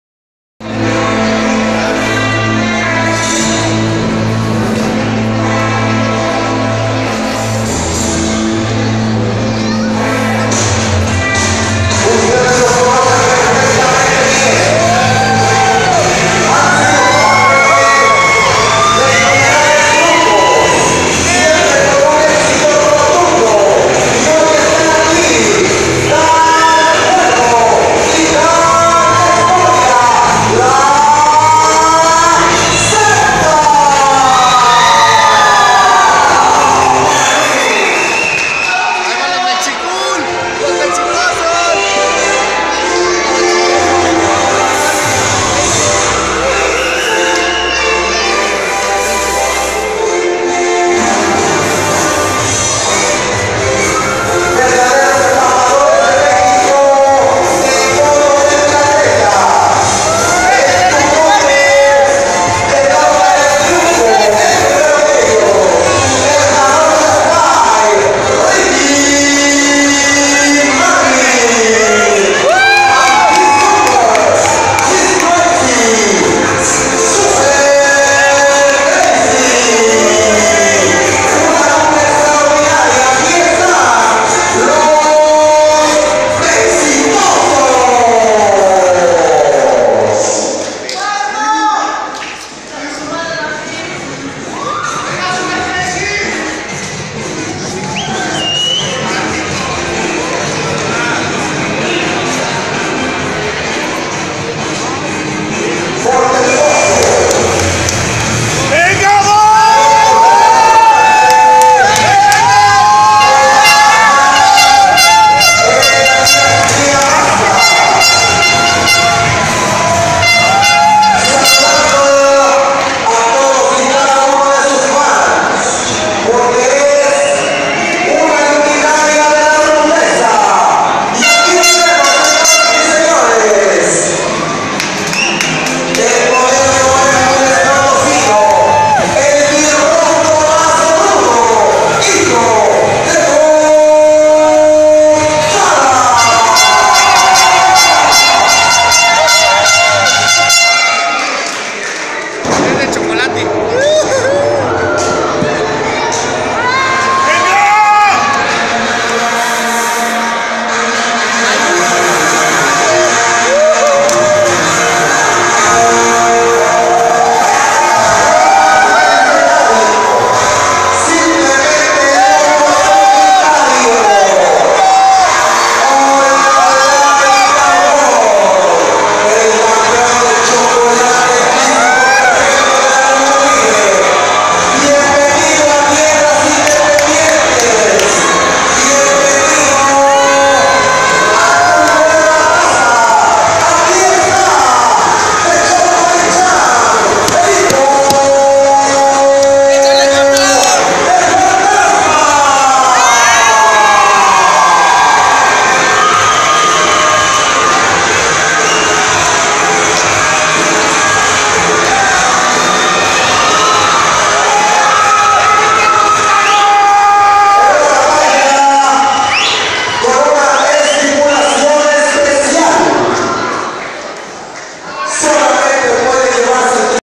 WR PRESENTO ESTA GRAN LUCHA !!!
Un triangular de Parejas con Los Mexitosos vs. La Secta vs. Hijo del Fantasma e Hijo de Dos Caras.